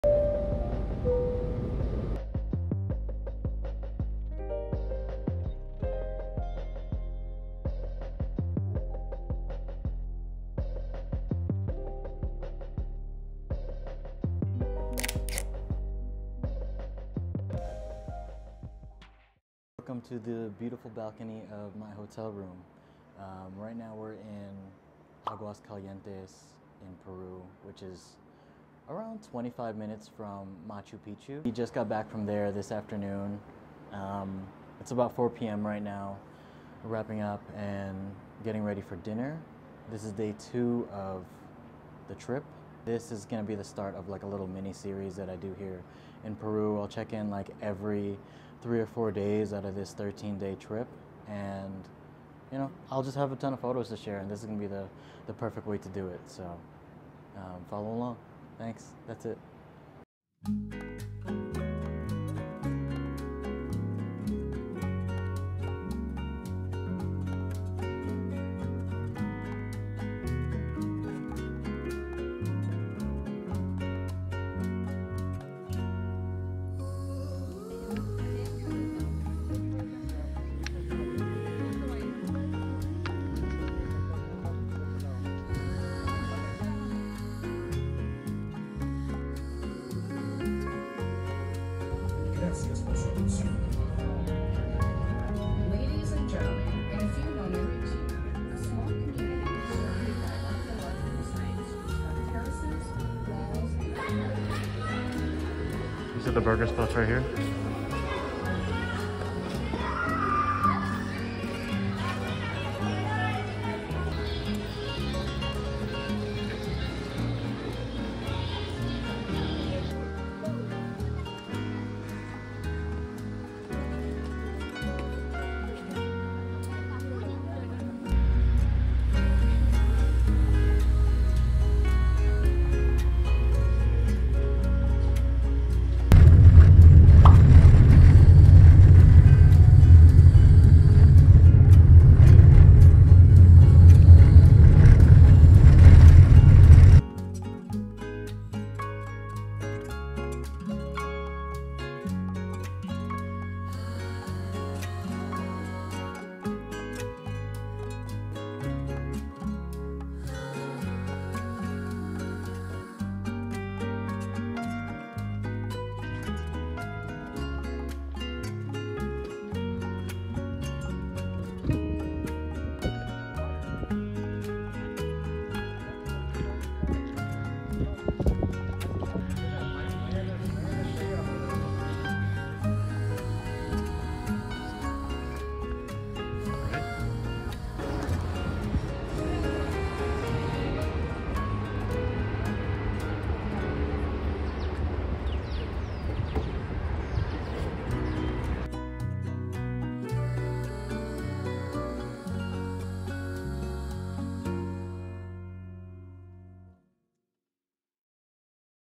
My first time in Peru | A Photography Vlog
Documenting my adventures through Peru on this 13-day trip with my partner. As mentioned in the video, I will be breaking this up into 2-3 parts to make it more digestible. We had a pretty jam-packed itinerary for the first leg of this trip, basically traveling to a new city every day.

In this video, we started in Cusco after about 8-hours of travel. We immediately hopped on a train the next morning to check out Macchu Picchu and Huayna Picchu. Afterward, we took another train to Ollantaytambo for a popular multi-stop tour around the Sacred Valley.

We then went back to Cusco for a few more days to finish the Rainbow Mountain trek (which wasn't shown in this video because it was so intense!)

At the end of the video, I teased the next video, which takes place in Lima!  Hope you enjoyed it!

Links to some of my gear:
Leica SL2-S Starter Bundle
Panasonic 85mm 1.8 (L-Mount)
Canon G7XIII
Kodak Tripod
Leica M10
28mm f2,8 Carl Zeiss Biogon ZM